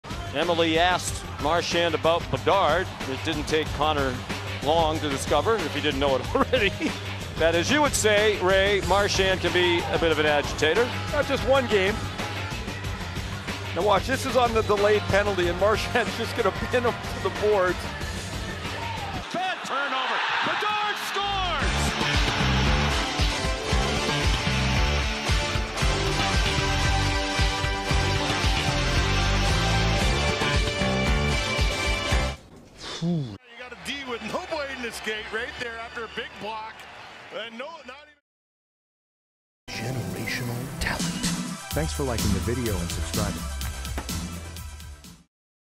Connor Bedard Gets Trolled by Bruins Marchand
...And he took that personally.